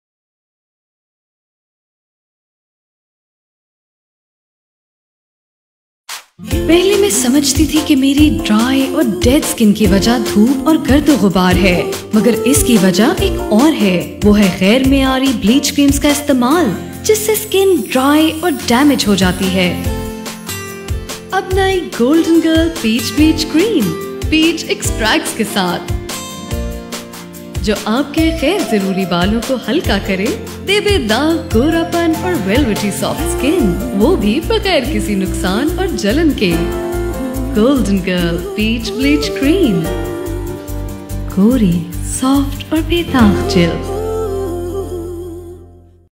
Golden Girl Peach Cream Bleach tvc
Golden Girl is a local skin care brand manufactured by GoldenGirl Cosmetics. Overall the brand is growing steadily and getting appreciation due to its good quality.

Client Name: GOLDEN GIRL
Director: Babar Zaffar
Production: Sea Films